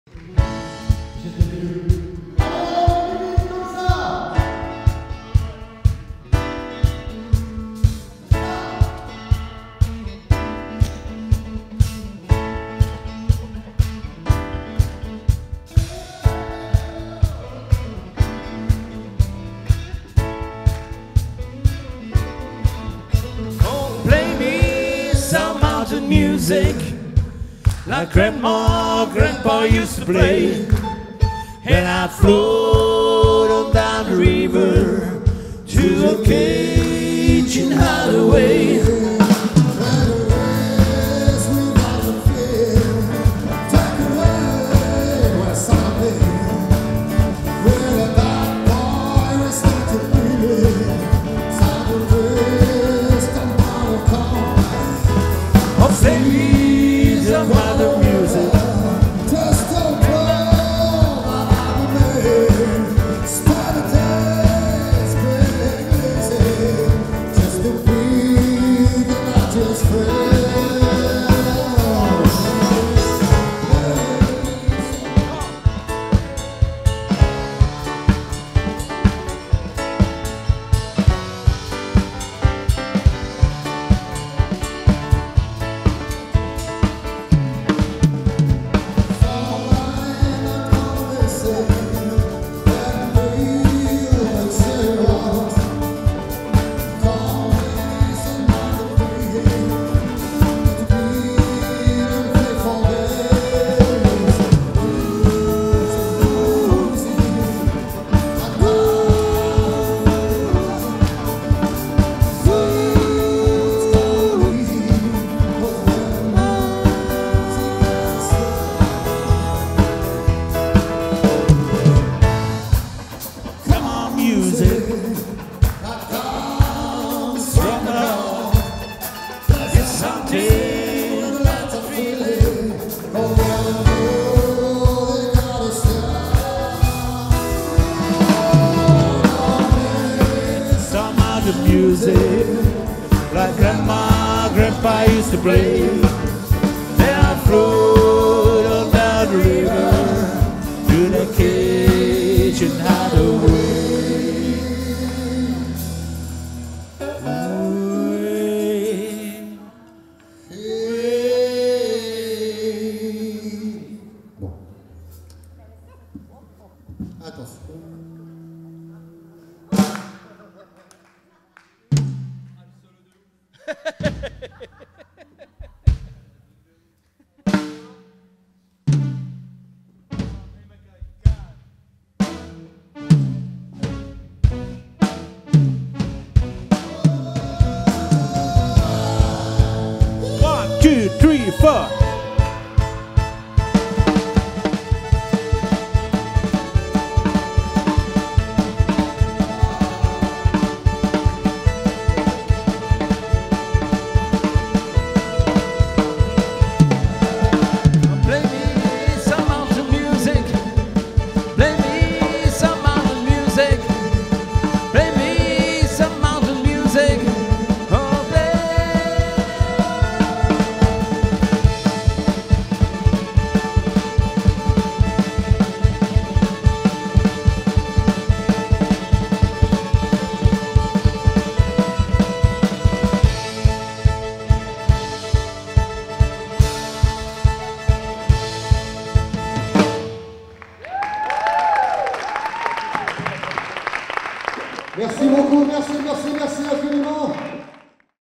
Les "Honky Tonk Farmers" en concert le samedi 15 mars 2014 à Limours (Essonne)
Pierre Lorry avec les "Honky Tonk Farmers" samedi 15 mars 2014 à Limours (Essonne)